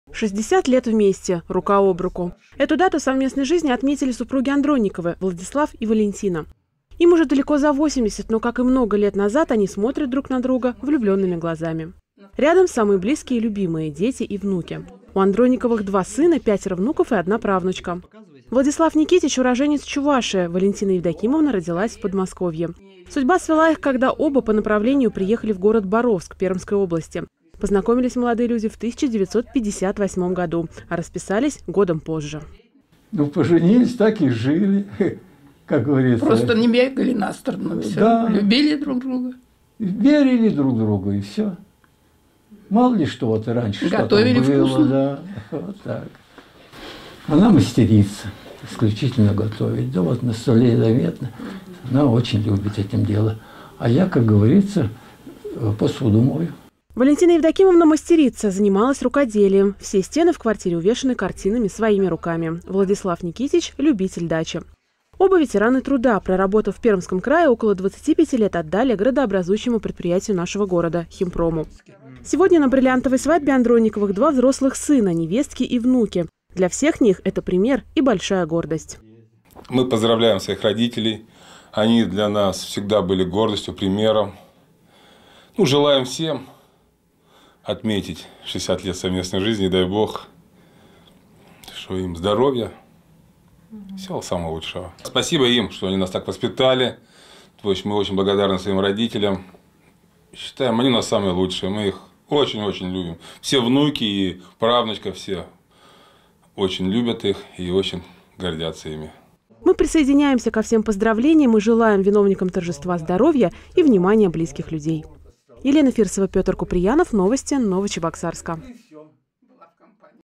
Семья Андронниковых: 60 лет рука об руку
Бриллиантовая свадьба – это целых 60 лет жизни в браке. Такой юбилей еще называется алмазной свадьбой. Бриллиант – это самый ценный и дорогой из всех камней. Также как и супружеская жизнь, просуществовавшая 60 лет, очень редка и ценна. На днях наша съемочная группа побывала на праздновании шести десятков лет совместно прожитой жизни у семьи Андронниковых.